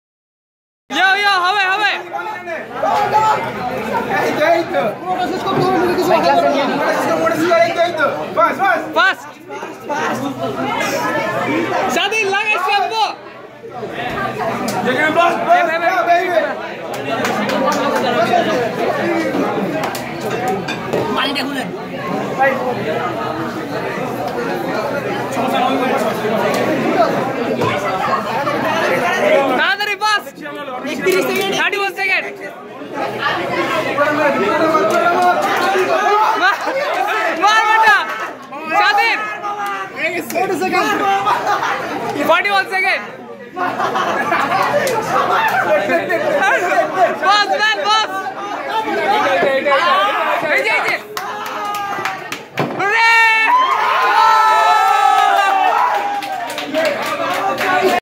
Pizza challenge in 90 second at Dhanmondi || Bunkers Cafe & Restaurant || completed at 48 seconds
90 Seconds to Eat the Pizza. 10 inch pizza challenge at dhanmondi. Shadhin and linkon complete it at 48 second.